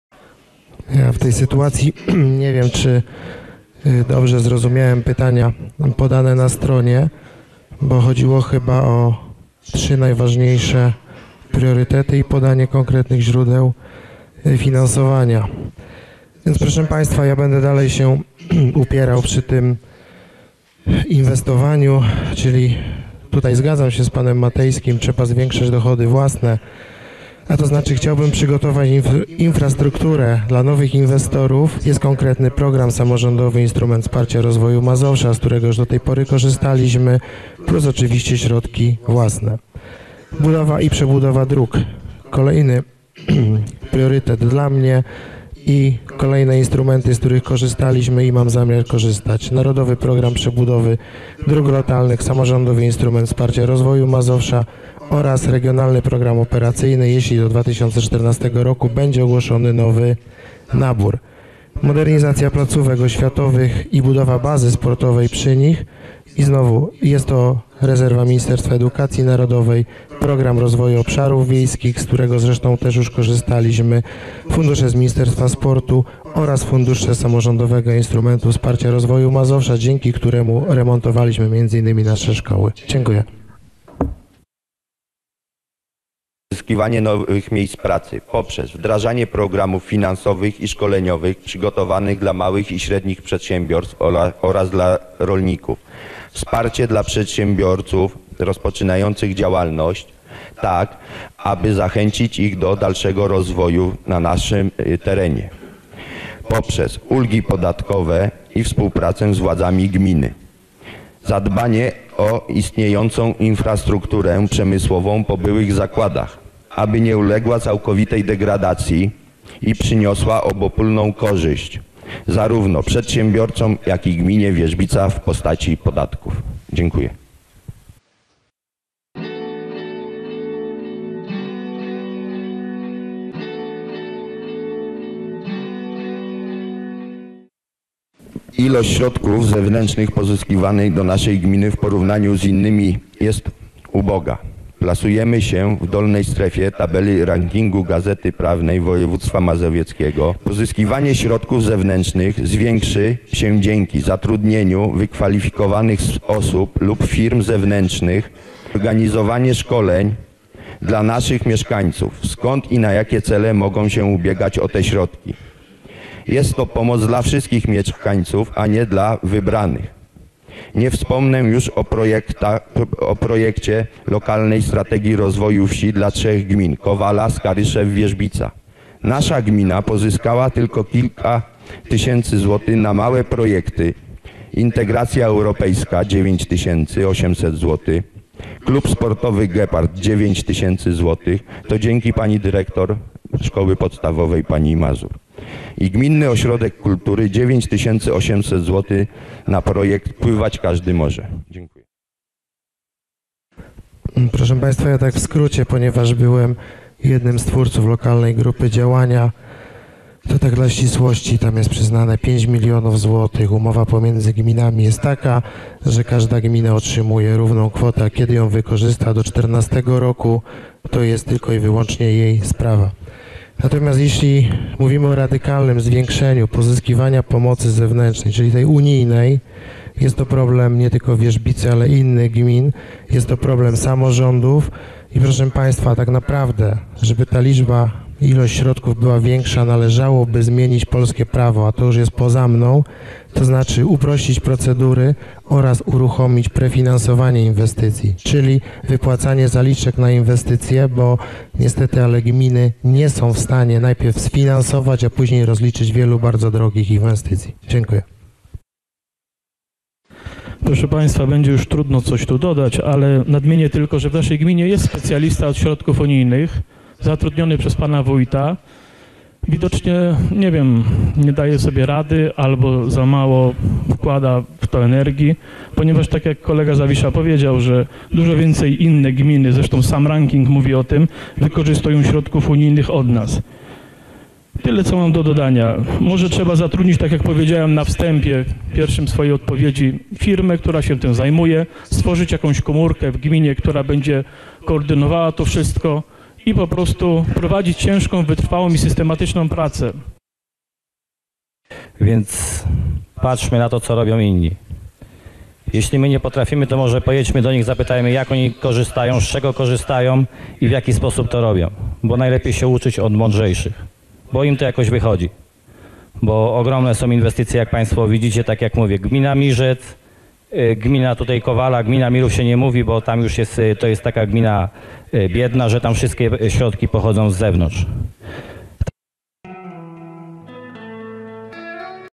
Skrót wideo debaty wierzbica.net 2010 - część 3
Skrócony zapis wideo debaty kandydatów na wójta gminy Wierzbica, zorganizowanej przez portal mieszkańców Wierzbicy w dniu 15 listopada 2010 roku.